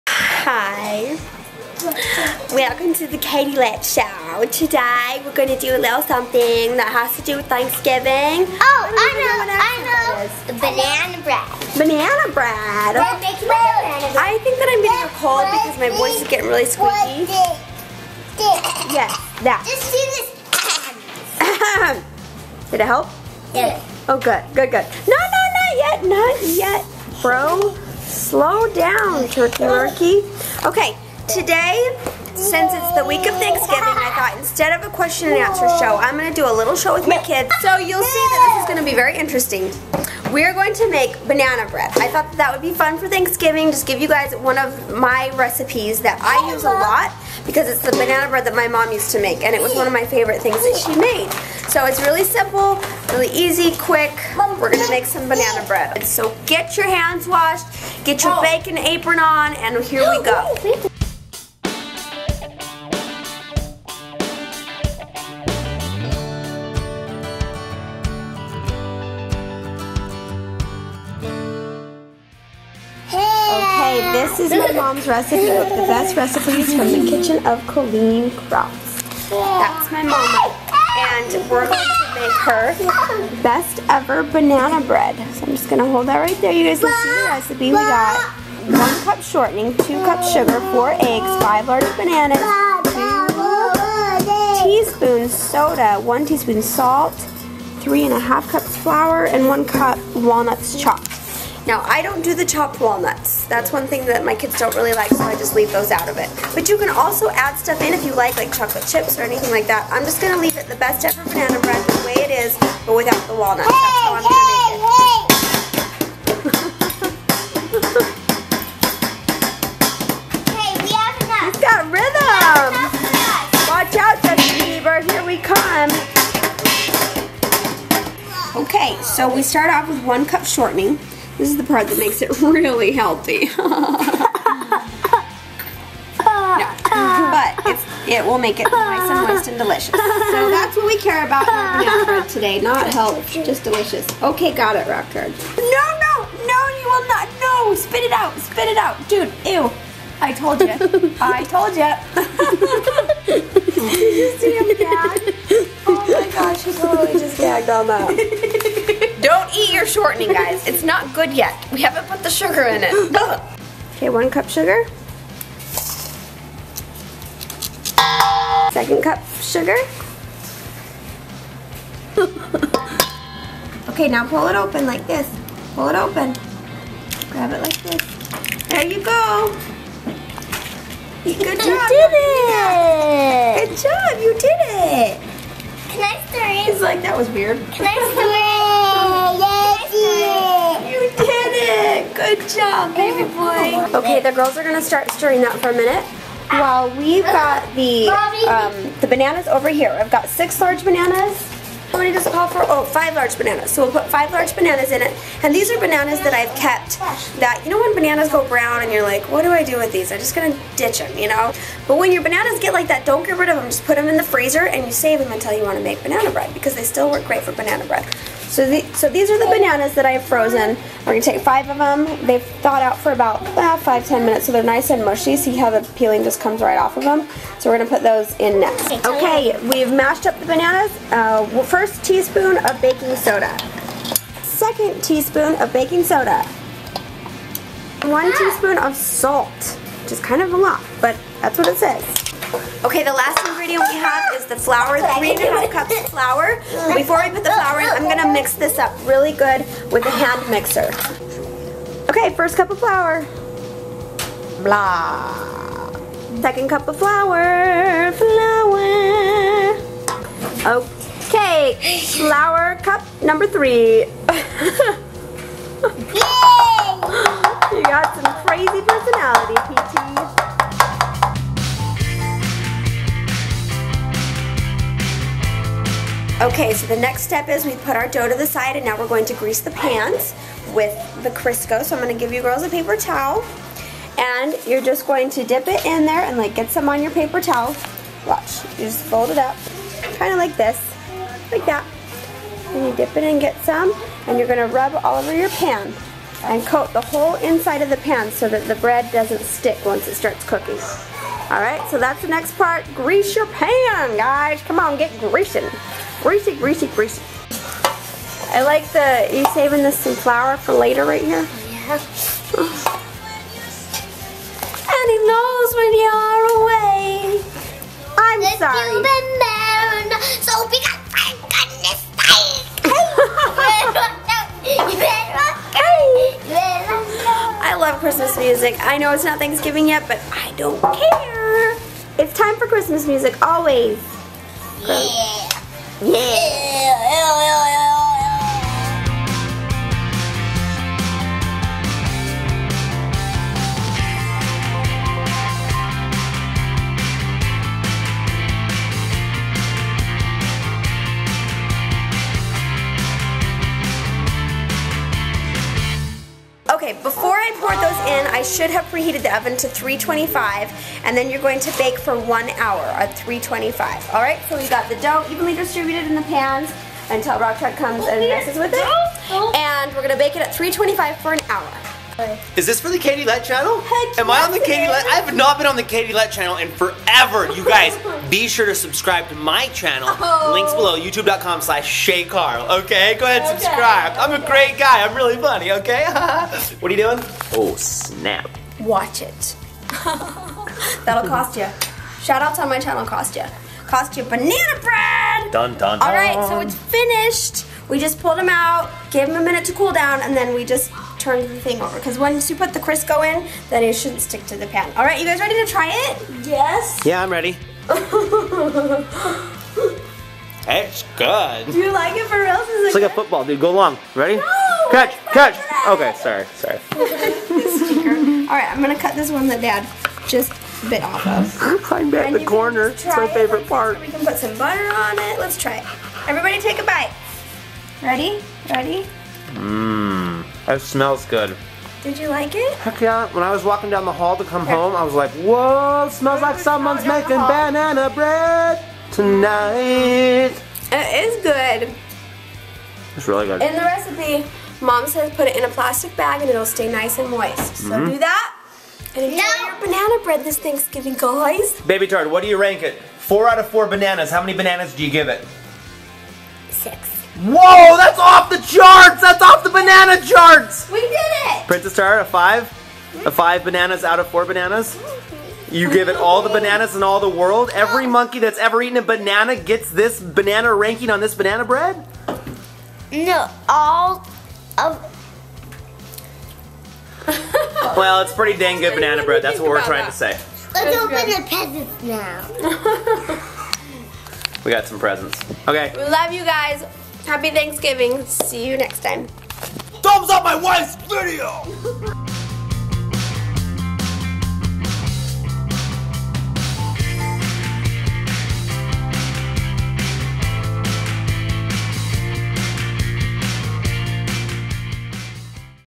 Best Ever Banana Bread!
This recipe is my mother's. :) I have always loved it and now I get to share it with you! Please let me know how it turns out! Enjoy!!!